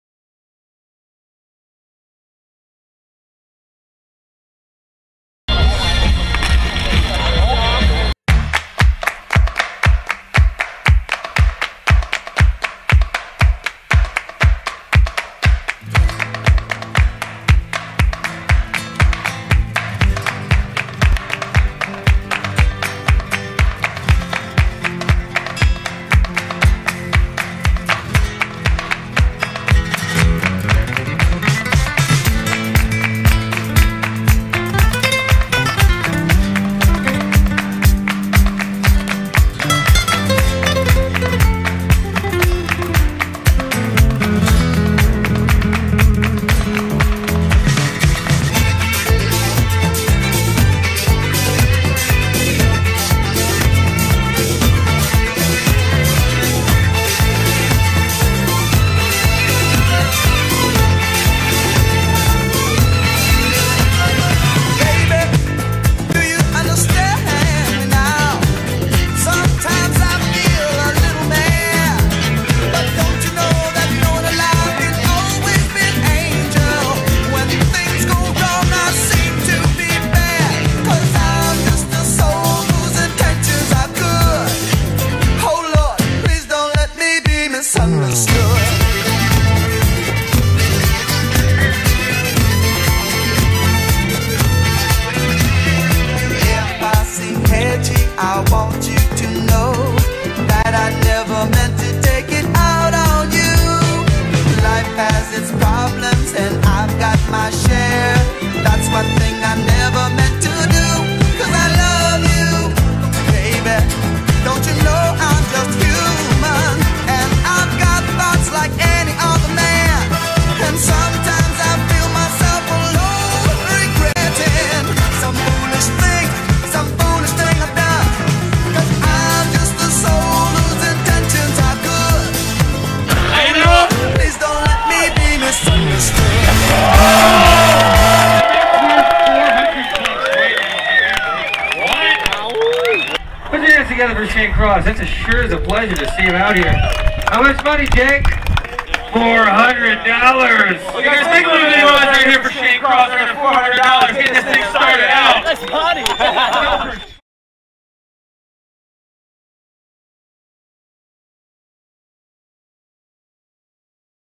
SHANE CROSS MURAL
FLEW TO MELBOURNE TO CELEBRATE DANE BURMAN BEING AUSTRALIAS SKATER OF THE YEAR. CREATED A MURAL WHILE THERE